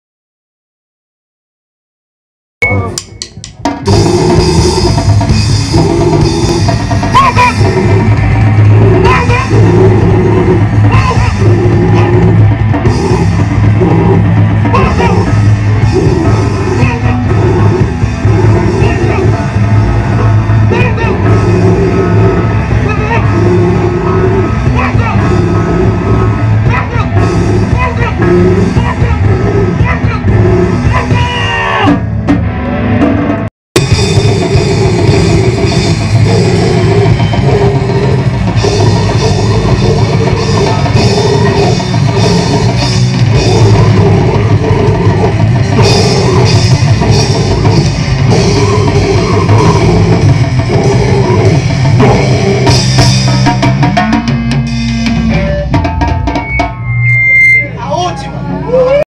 Monstromorgue @ Delmiro Gouveia - Fortalecendo o Grito dos Excluídos (set/2011)
A banda alagoana Monstromorgue se apresenta durante a GIG "Fortalecendo o Grito dos Excluídos" na cidade de Delmiro Gouveia - AL (10/09/2011).

Imagens: Gustavo Ataide
Edição: Gustavo Ataide